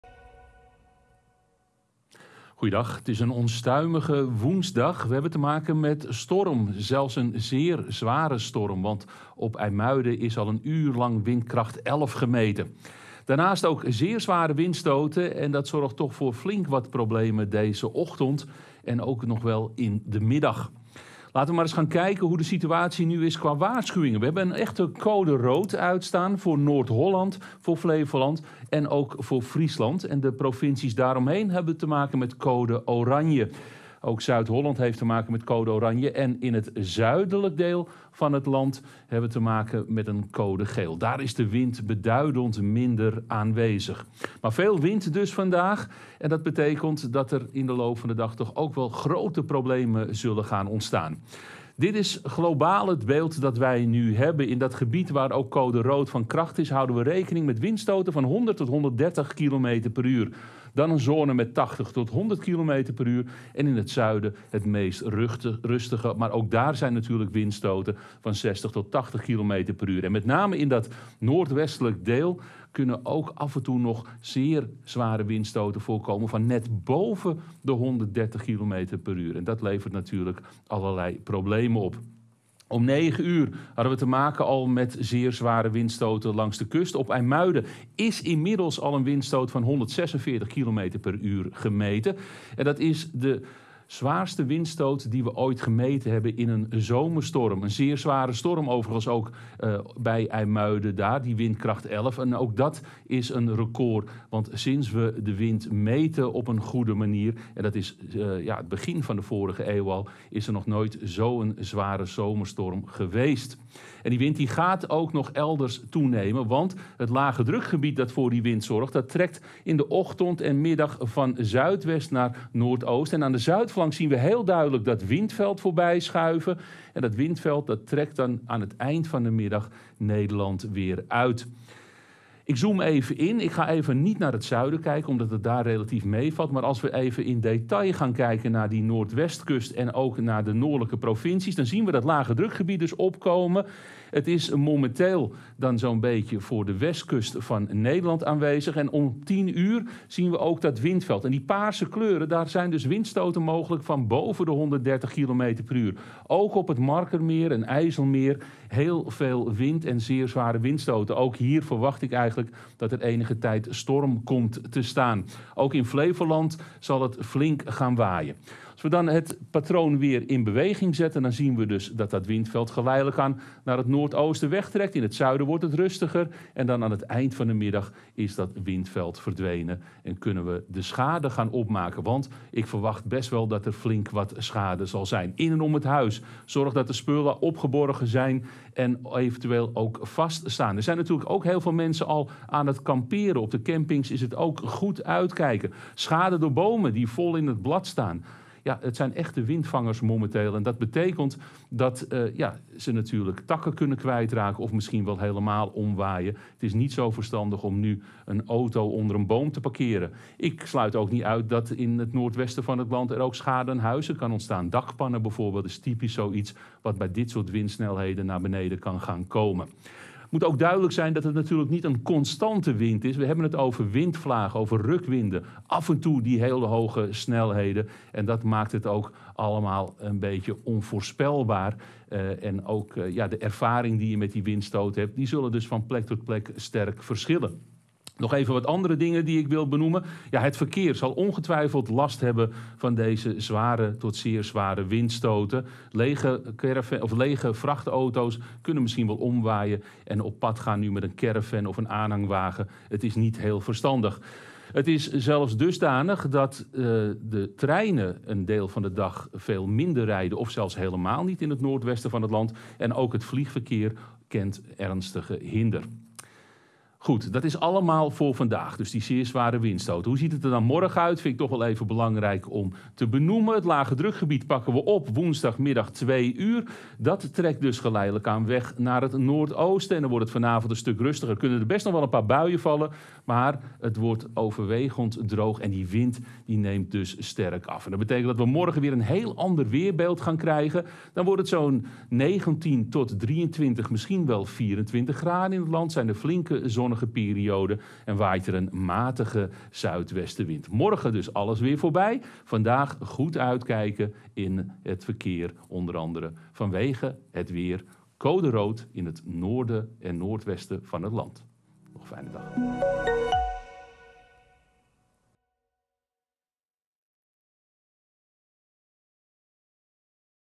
Extra weerbericht: zeer zware storm Poly trekt over Nederland!
Er geldt code rood voor zeer zware storm Poly. Er zijn windstoten mogelijk van 100 tot 130 km/uur. Raymond Klaassen vertelt je alle details!